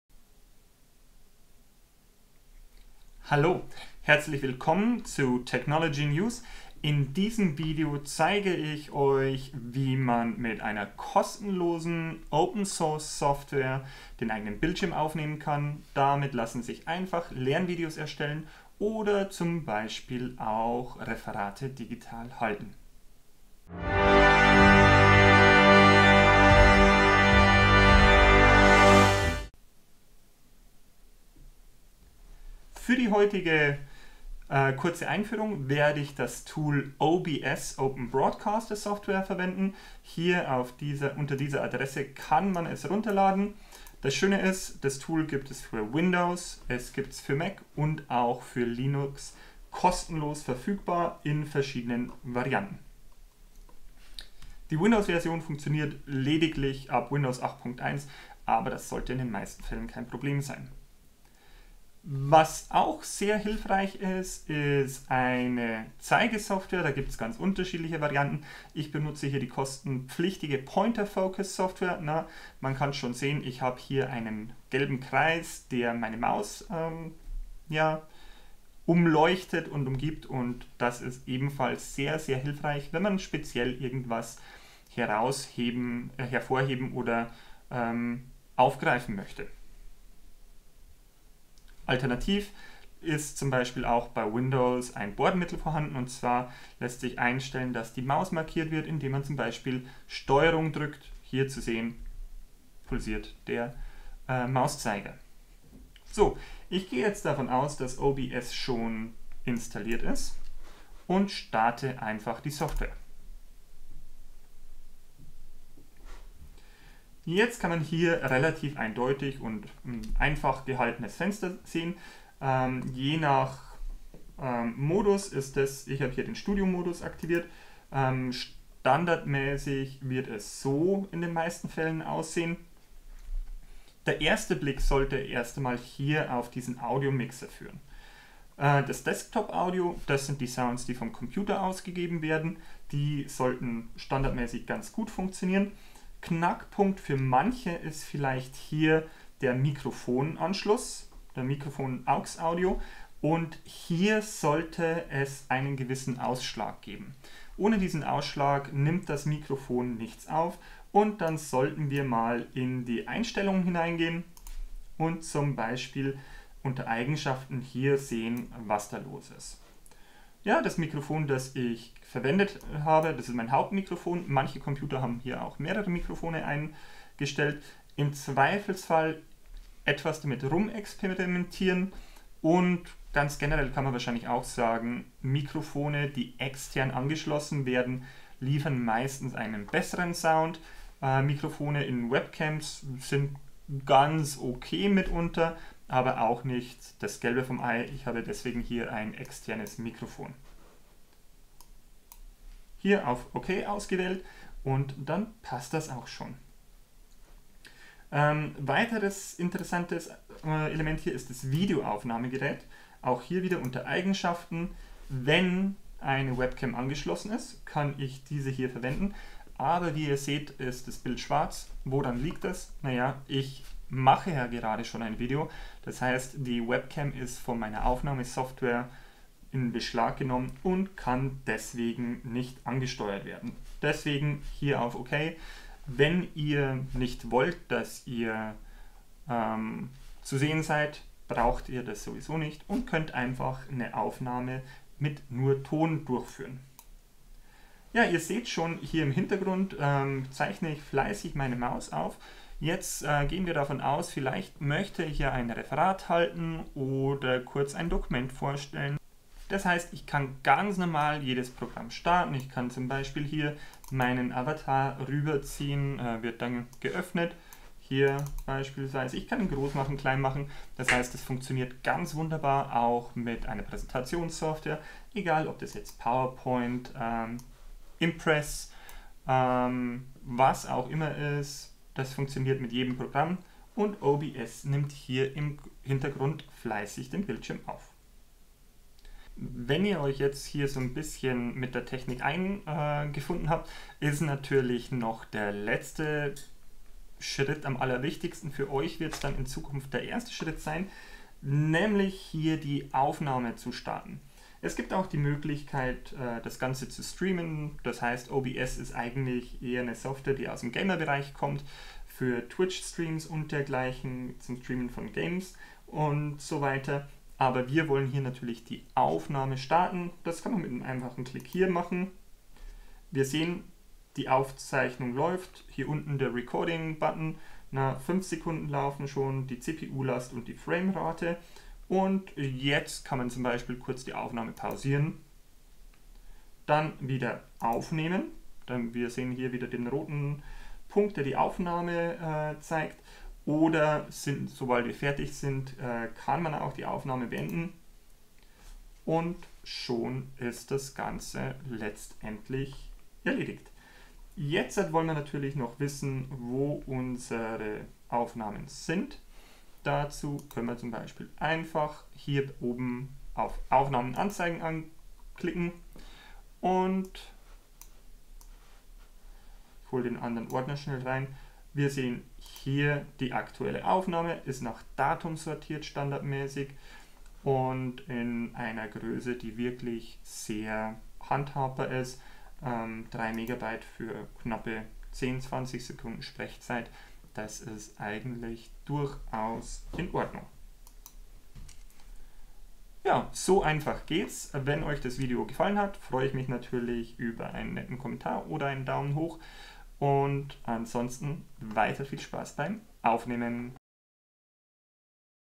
Bildschirmaufnahme mit OBS - Screen Recorder
Dieses Video zeigt, wie sich ganz einfach mit der kostenlosen Software OBS ein Bildschirmvideo erstellen lässt, mit dem Inhalte präsentiert oder Referate digital gehalten werden können.